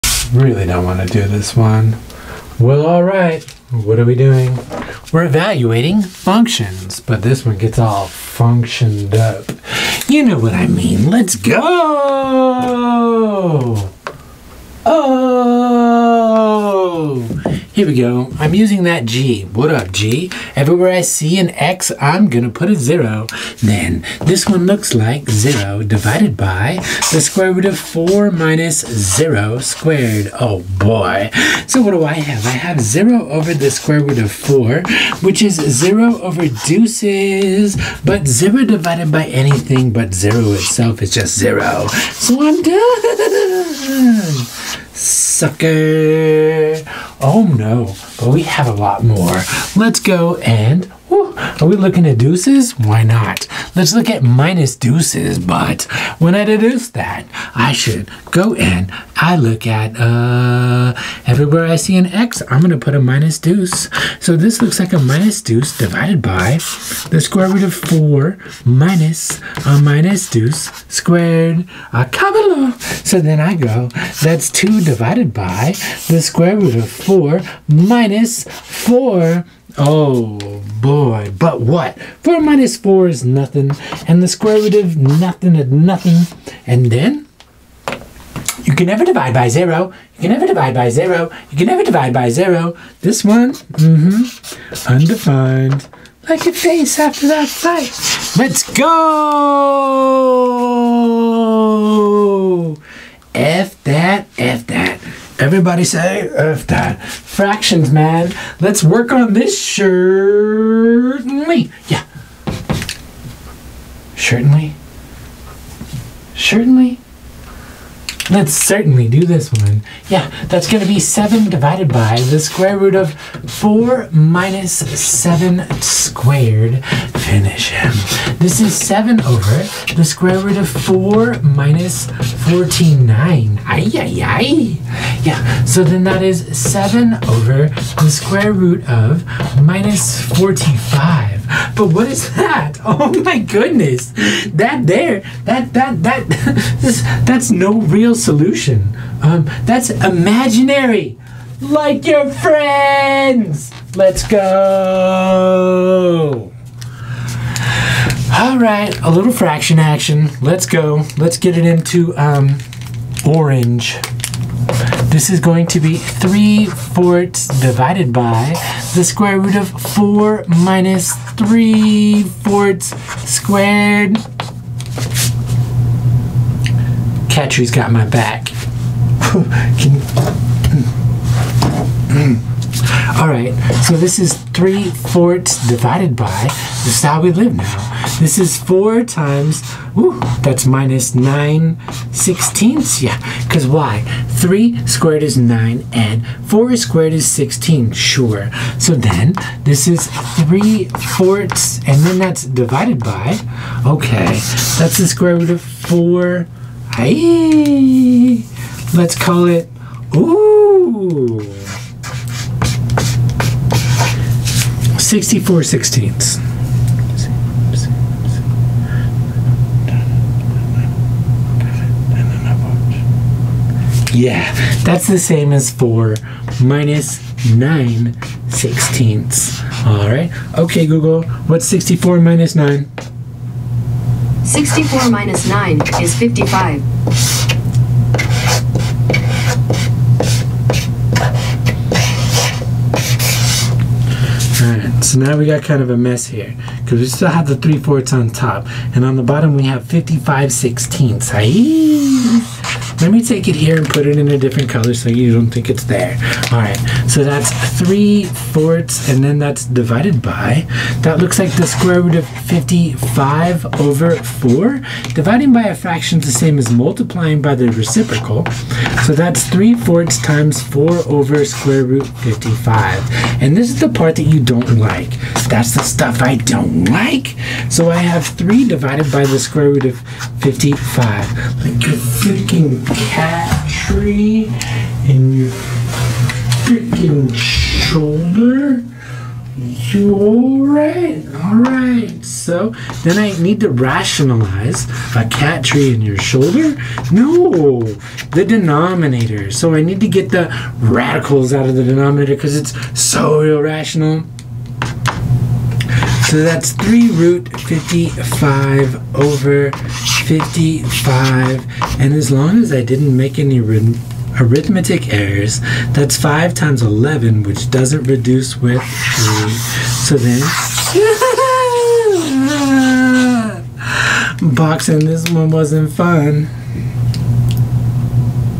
Evaluating Functions with Radicals and Fractions 4 Exciting Examples with Exotic Solutions.
This video is for intermediate and college algebra courses. The function has fractions and radicals. This flick explores what happens when you get  Undefined, Imaginary, Rational and Irrational solutions after evaluating the function.